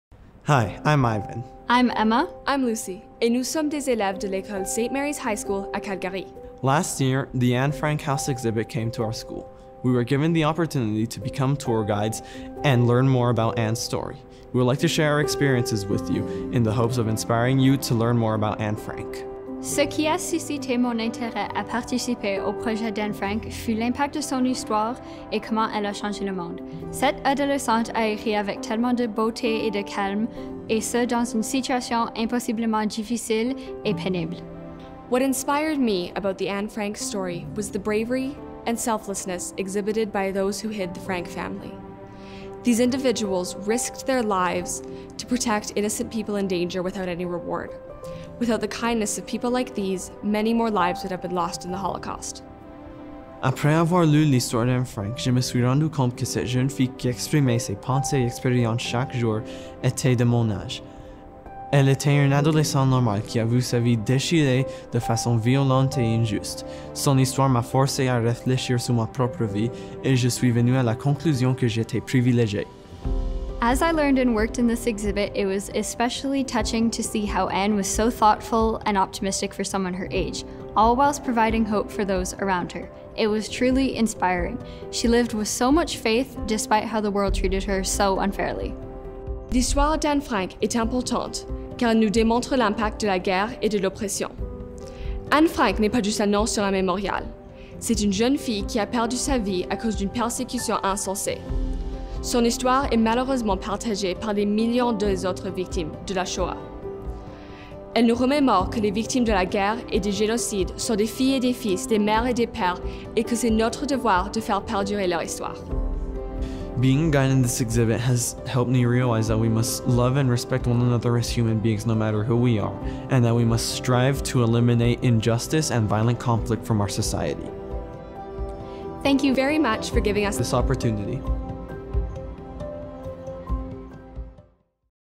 Anne Frank - A History for Today | St. Mary's High School, Calgary
Before it travelled to the Canadian War Museum, Anne Frank – A History for Today was presented in Calgary in early 2020. In this video, three students from St. Mary’s High School  share their thoughts on what the story of Anne Frank meant to them.